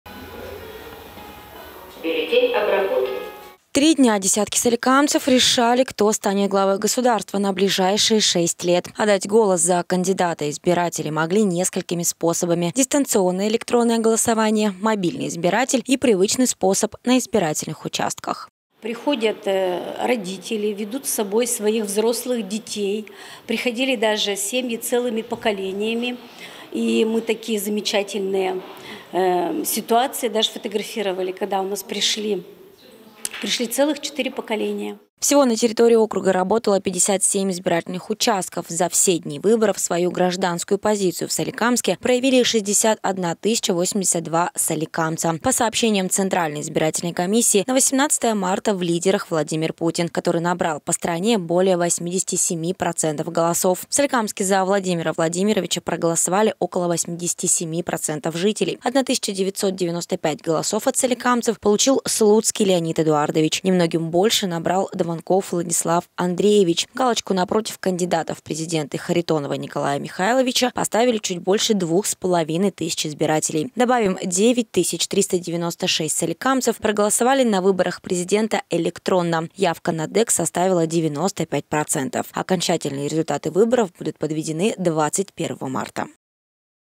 Выбор сделан_18.03.2024_СольТВ
На протяжении 3 дней жители всей России были объединены восьмыми выборами президента. Сколько соликамцев выразили свою гражданскую позицию и за кого отдали свой голос, смотрите в этом материале.